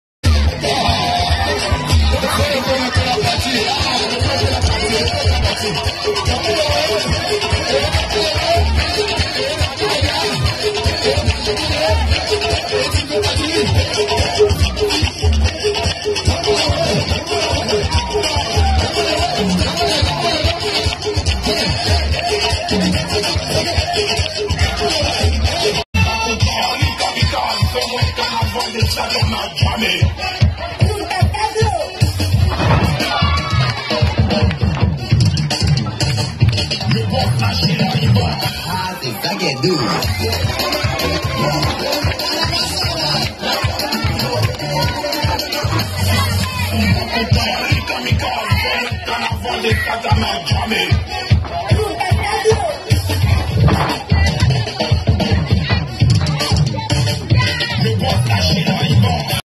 CLIC J'AIME MAIS LE FEU🔥 À EPV RAM
Une prestation de feu 🔥 et de joie grand merci au père RAM-TSR et au parrain ADEBAYOR LE SILENCIEUX

Fierté de sikensi Kaiba karaba